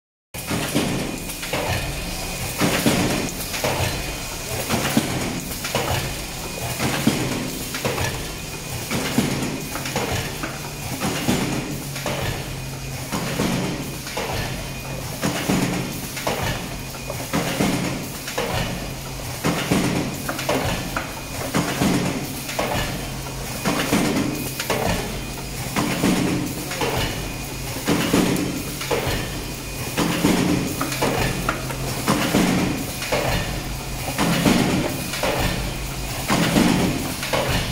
Paper food tray take away containers forming Machine
Paper food tray take away containers forming Machine, JBZ-800 paper lunch box heating sealing machine